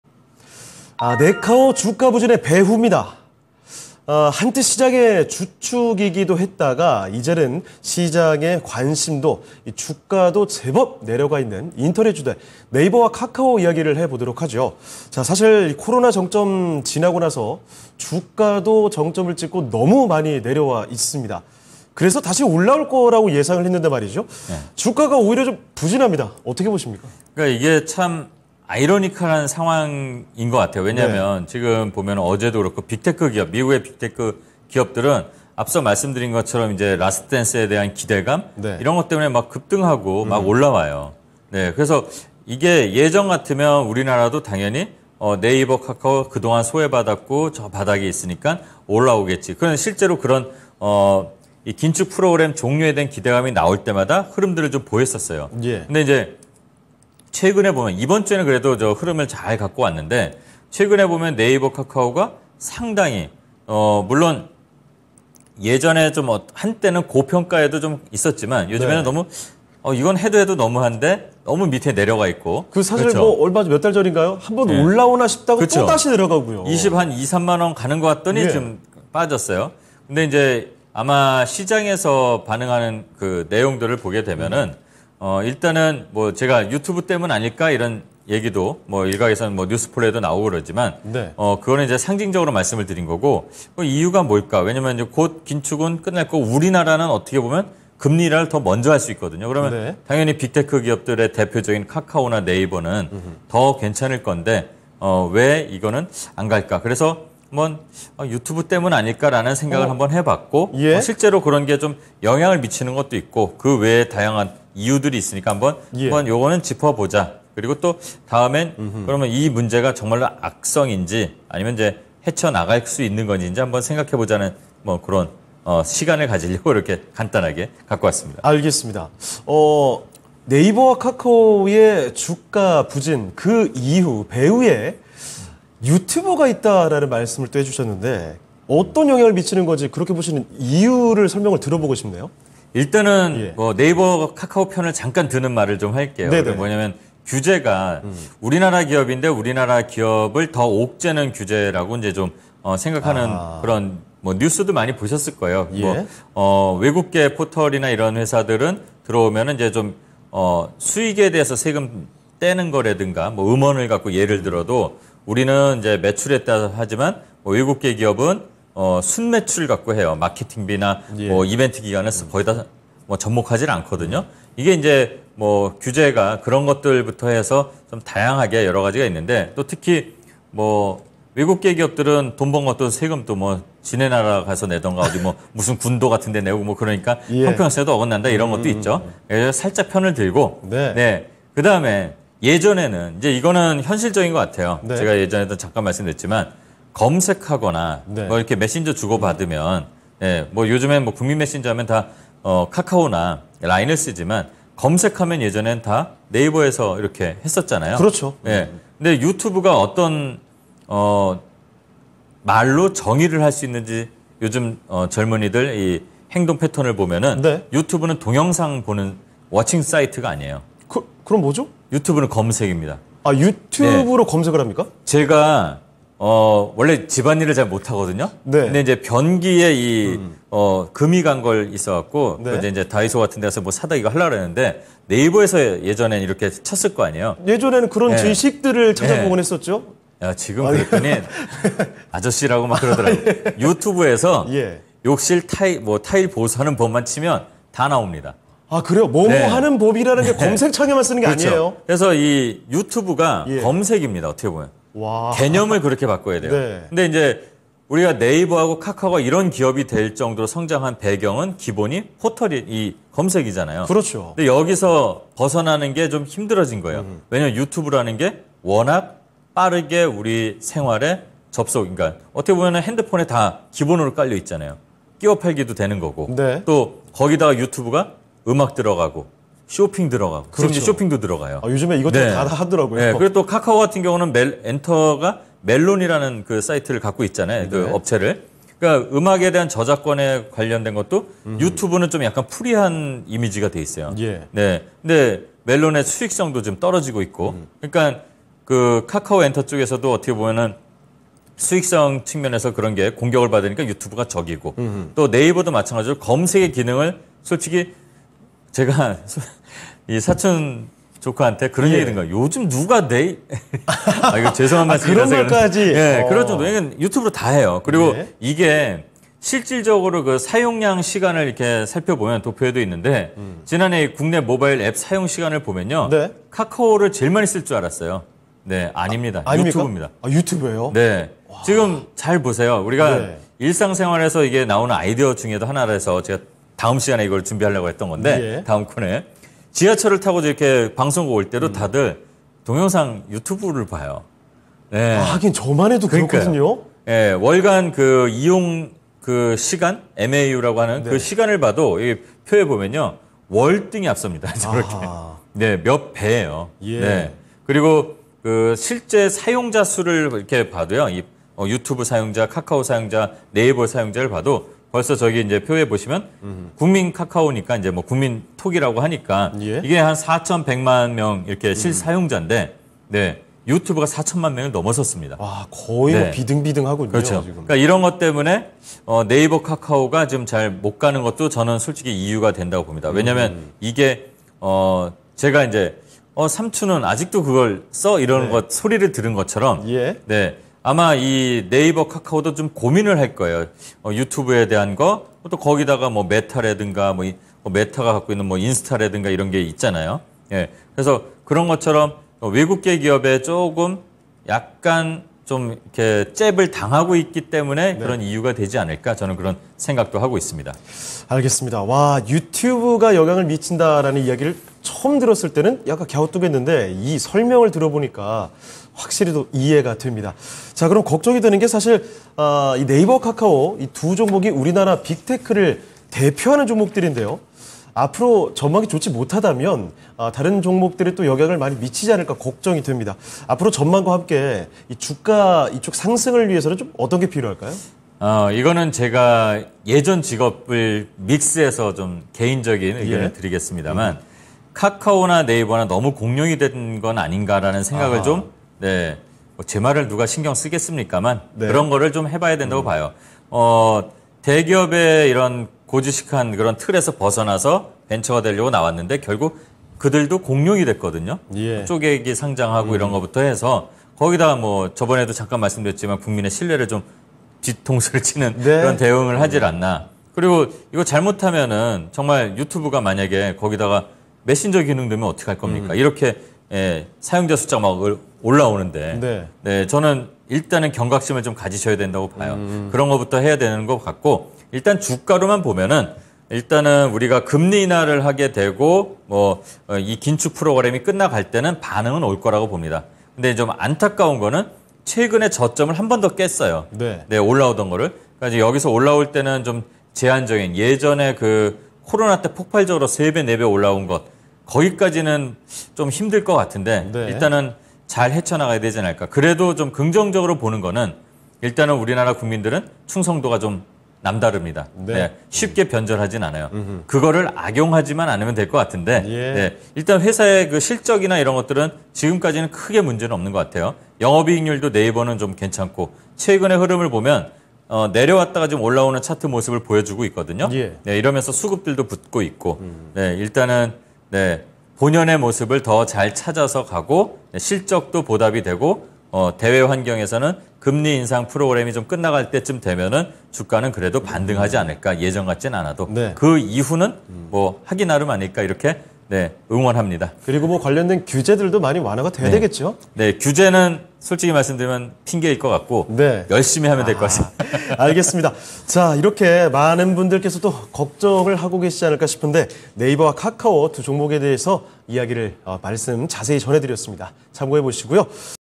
K-빅테크 '네카오' 부진은 역차별·유튜브 때문? [위클리 투자 노트 2]
▶위클리 투자 노트: 본방송 금 18:00~19:00
▶위클리 투자뉴스② 네카오 주가 부진의 배후
Q. NAVER/카카오 업황 흐름은?
Q. 네카오 주가 부진, 유튜브 때문?
Q. 'K-빅테크' 네카오, 향후 전망 및 상승 조건은?